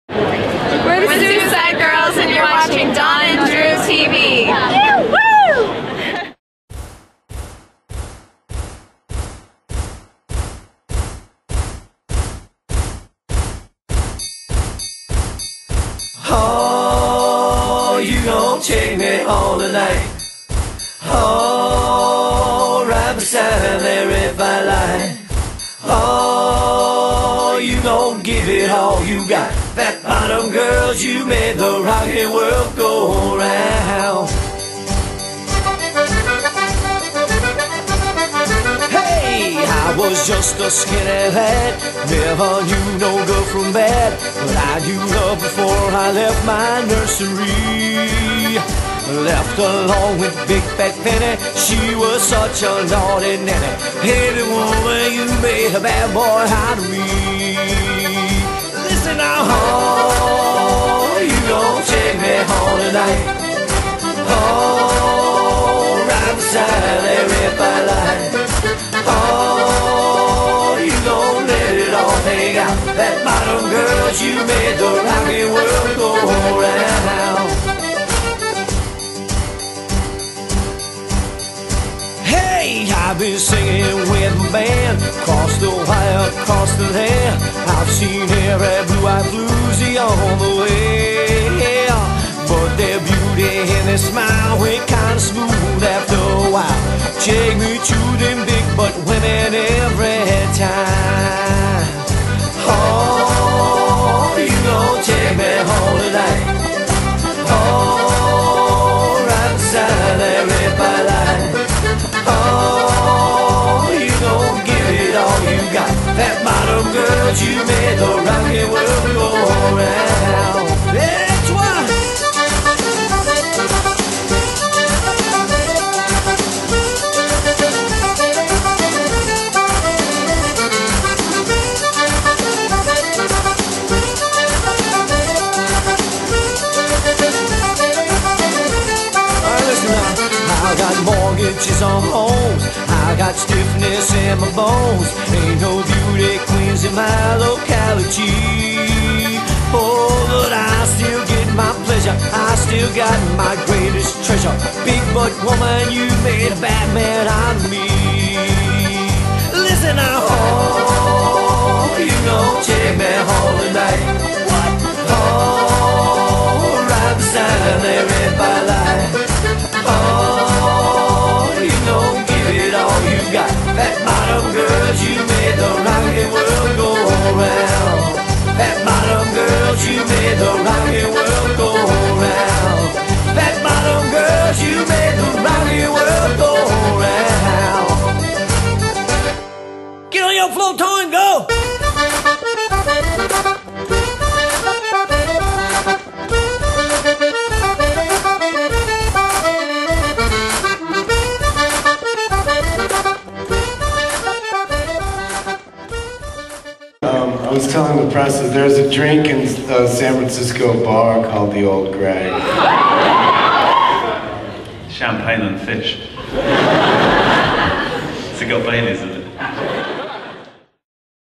D&Dtv - Comic Con Dance
thanks to our roving D&Dtv correspondents, Robby, Nataly and Sal, we had a presence at this year's Comic-Con, and Nataly made sure to get her groove on with a lot of the costumed attendees. there's even a minion sighting with ebilflindas!